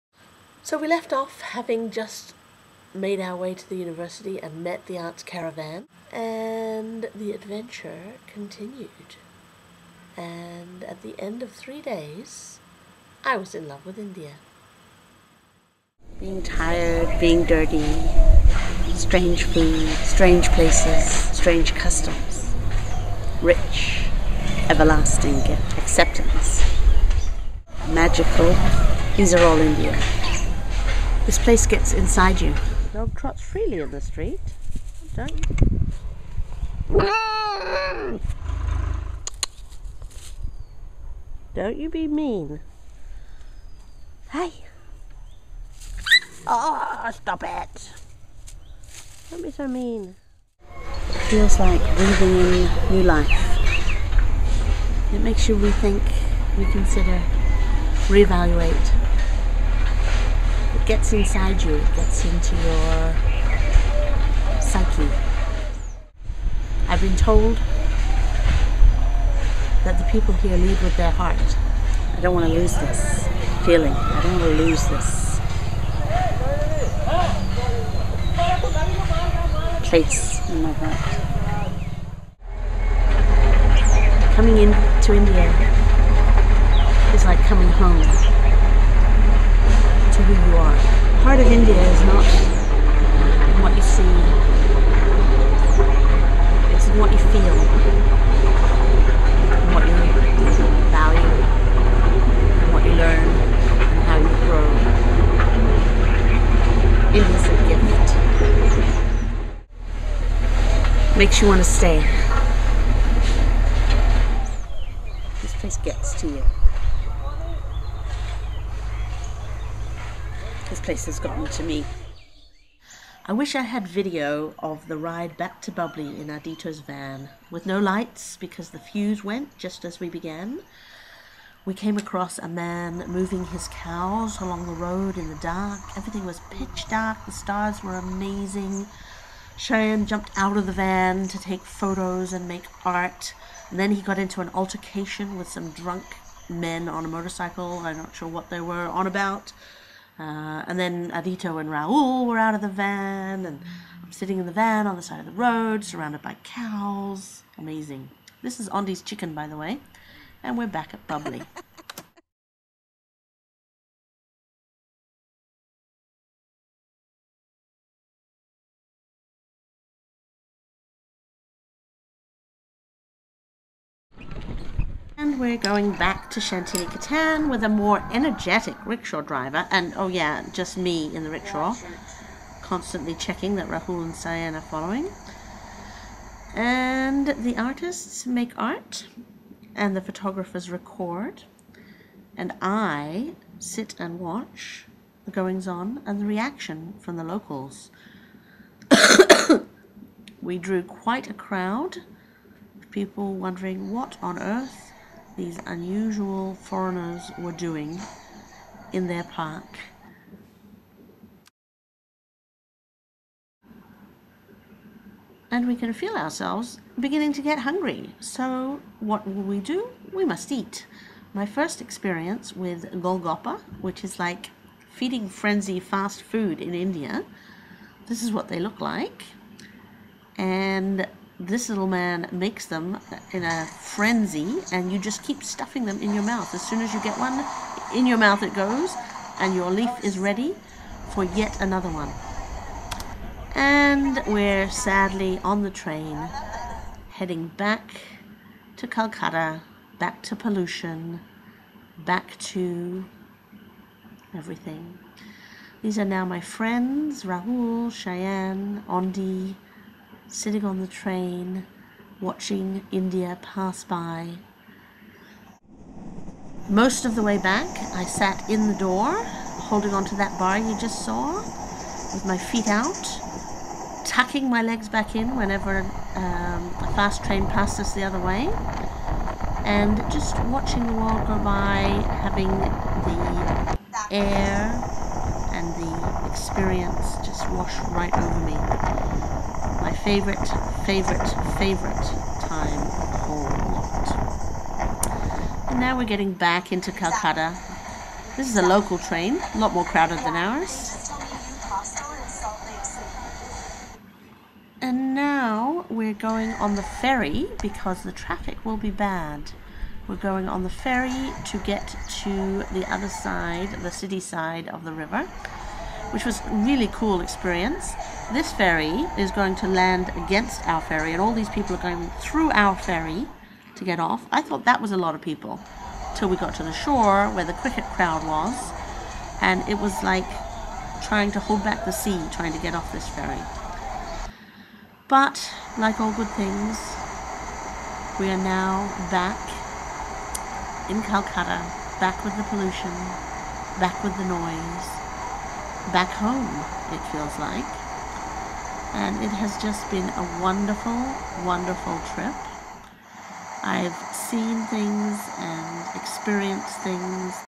Shanti Adventure - Part 2
More of the trip that taught me to love India!
But so many things I forgot!
The food!, the social life, the mandolin and singing, the rum.
Ask me when you see me!